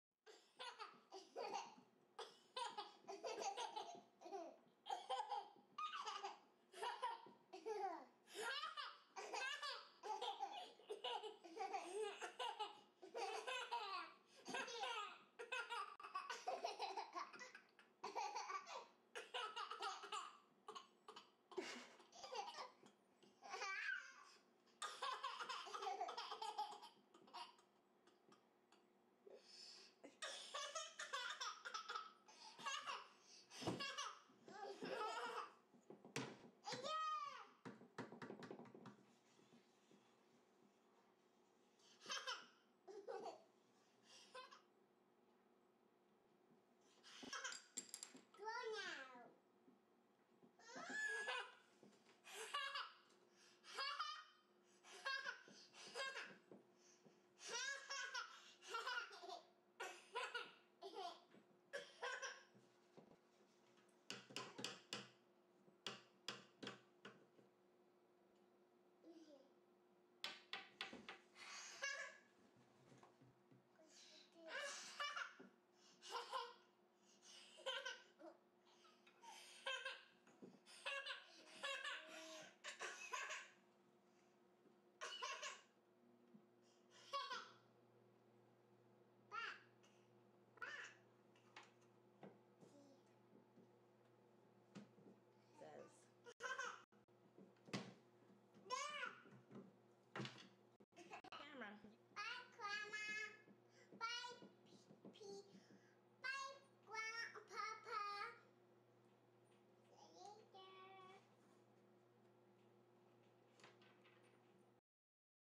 Laughing Children at the Drawing Table
Aureilia laughing with her friend...whether he likes it or NOT!  Draw now!  Back!  Back!